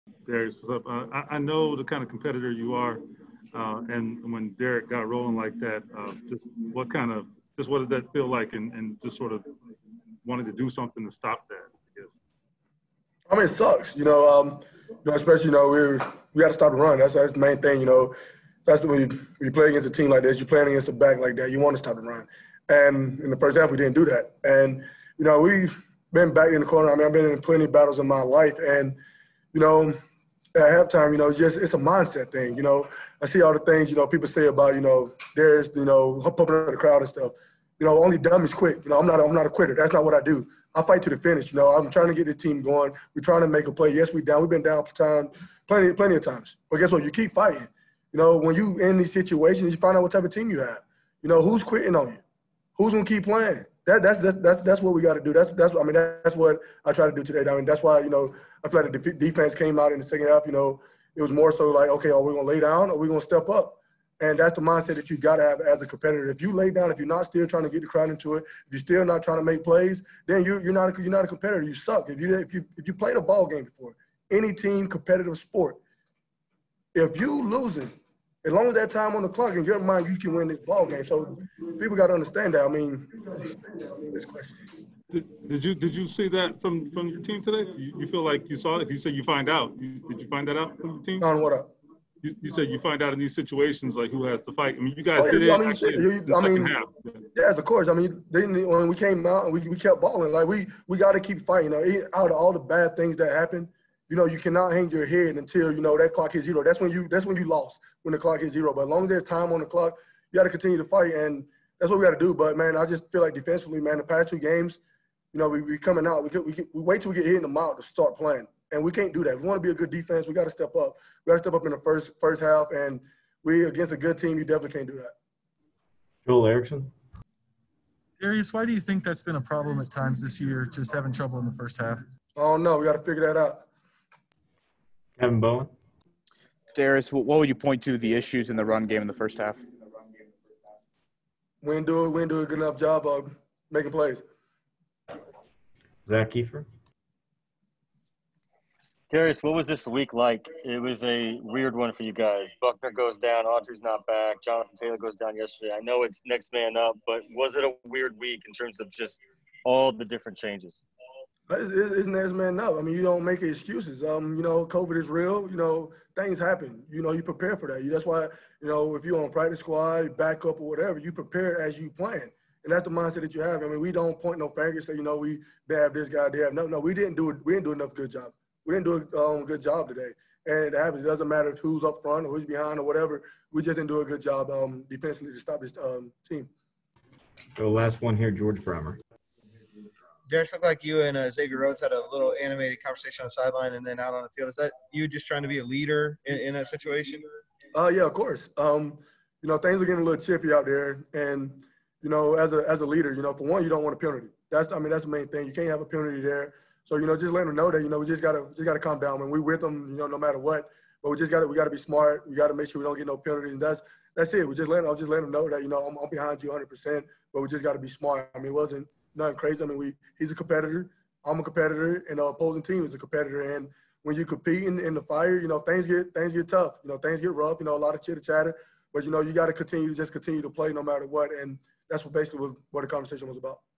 Darius Leonard On Continuing To Fight, Titans Run Game
Indianapolis Colts linebacker Darius Leonard spoke with the media about how he and the entire defense continued to battle throughout the entire game and how the Tennessee Titans and Derrick Henry were able to have success in the run game.